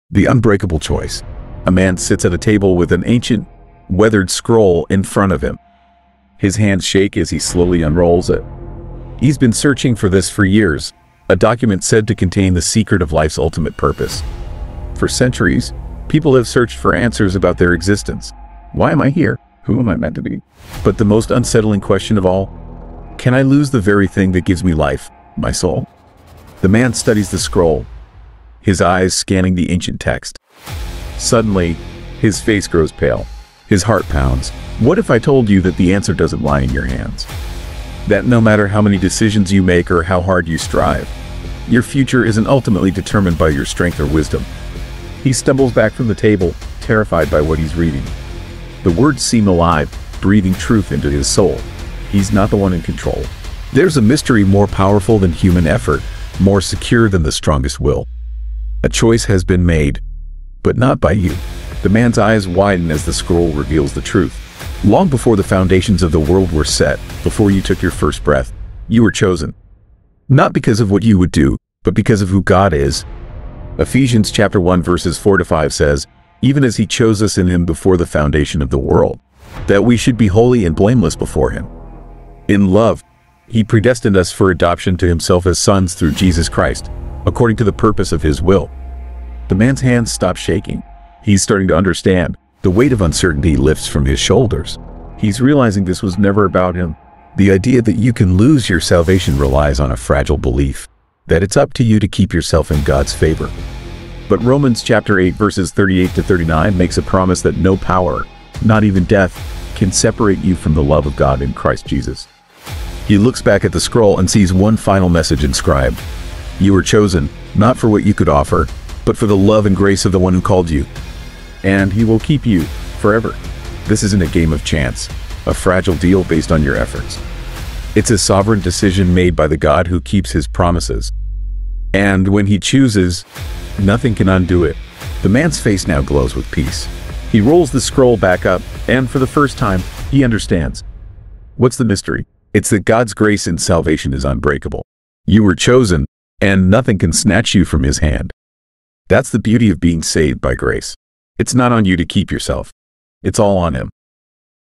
Best Kept Secret to Eternal Security EXPOSED!
The mystery of eternal security

Sure, here’s the text of the mystery short without the extra narrative cues:

**The Unbreakable Choice**

A man sits at a table with an ancient, weathered scroll in front of him. His hands shake as he slowly unrolls it. He’s been searching for this for years—a document said to contain the secret of life’s ultimate purpose.

"For centuries, people have searched for answers about their existence. Why am I here? Who am I meant to be? But the most unsettling question of all—can I lose the very thing that gives me life, my soul?"

The man studies the scroll, his eyes scanning the ancient text. Suddenly, his face grows pale. His heart pounds.

"What if I told you that the answer doesn’t lie in your hands? That no matter how many decisions you make or how hard you strive, your future isn’t ultimately determined by your strength or wisdom."

He stumbles back from the table, terrified by what he’s reading. The words seem alive, breathing truth into his soul. He’s not the one in control.

"There’s a mystery more powerful than human effort, more secure than the strongest will. A choice has been made, but not by you."

The man’s eyes widen as the scroll reveals the truth.

"Long before the foundations of the world were set, before you took your first breath, you were chosen. Not because of what you would do, but because of who God is. Ephesians 1:4-5 says, 'Even as He chose us in Him before the foundation of the world, that we should be holy and blameless before Him. In love, He predestined us for adoption to Himself as sons through Jesus Christ, according to the purpose of His will.'"

The man’s hands stop shaking. He’s starting to understand. The weight of uncertainty lifts from his shoulders. He’s realizing this was never about him.

"The idea that you can lose your salvation relies on a fragile belief—that it’s up to you to keep yourself in God’s favor. But Romans 8:38-39 makes a promise that no power, not even death, can separate you from the love of God in Christ Jesus."

He looks back at the scroll and sees one final message inscribed:

"You were chosen, not for what you could offer, but for the love and grace of the One who called you. And He will keep you, forever."

"This isn’t a game of chance, a fragile deal based on your efforts. It’s a sovereign decision made by the God who keeps His promises. And when He chooses, nothing can undo it."

The man’s face now glows with peace. He rolls the scroll back up, and for the first time, he understands.

"What’s the mystery? It’s that God’s grace in salvation is unbreakable. You were chosen, and nothing can snatch you from His hand. That’s the beauty of being saved by grace. It's not on you to keep yourself—it’s all on Him."

Proverbs
chosen path
God chooses
shorts